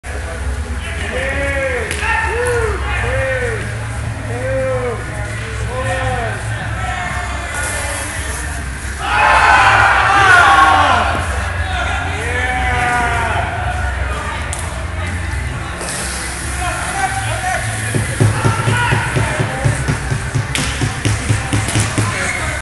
Awesomest broom-ball goal ever scored
Athletic phenom Wesley Seymour scores the winning goal for the Broom-Ball Championship of the World. From an amazing overhead pass, Wesley rains down a thunderous goal onto the unsuspecting goal keeper... who ever he may be.